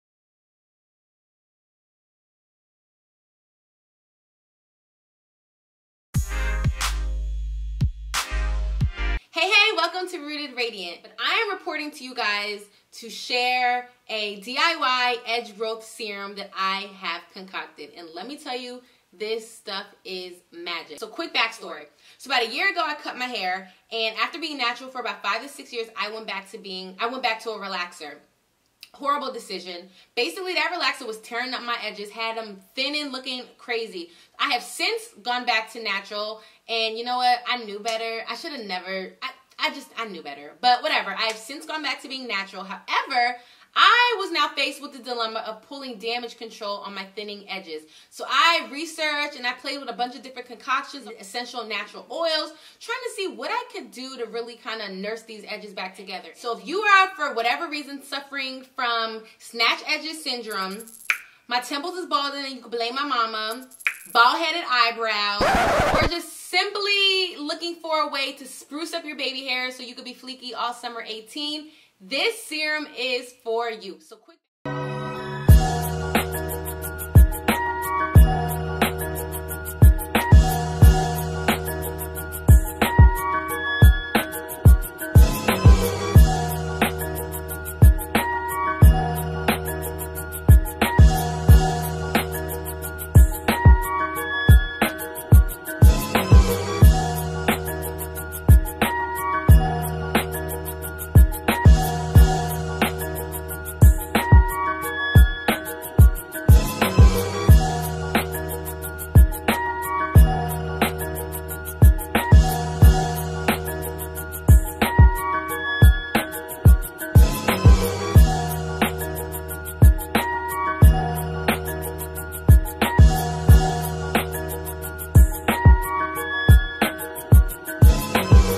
How to Make Edge Growth Serum | DIY
How to Make a DIY Edge Growth Serum

100% Natural & it REALLY WORKS.